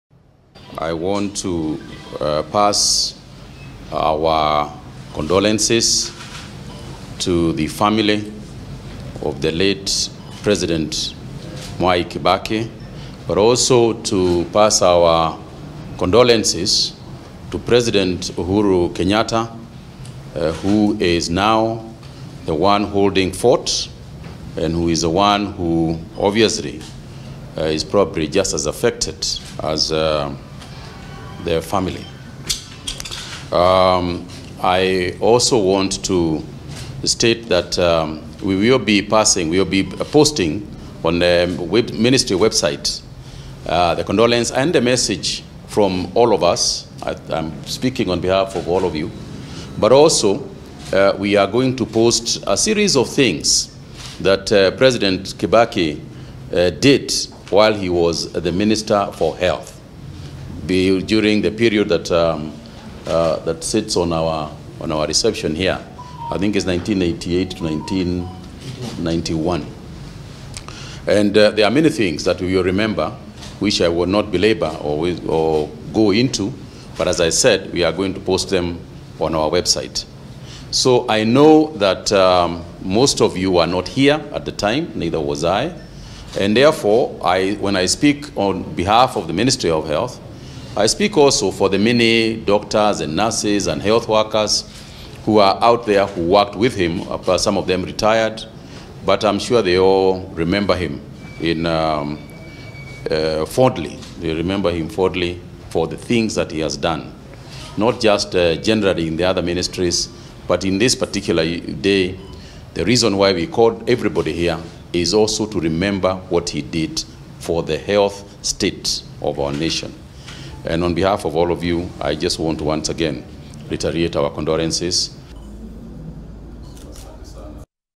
Health CS Mutahi Kagwe eulogizes Kibaki as a leader who accomplished much for Kenya
Health Cabinet Secretary Mutahi Kagwe has led the Ministry fraternity in the signing of the condolence book at Afya House following the death of former President Mwai Kibaki.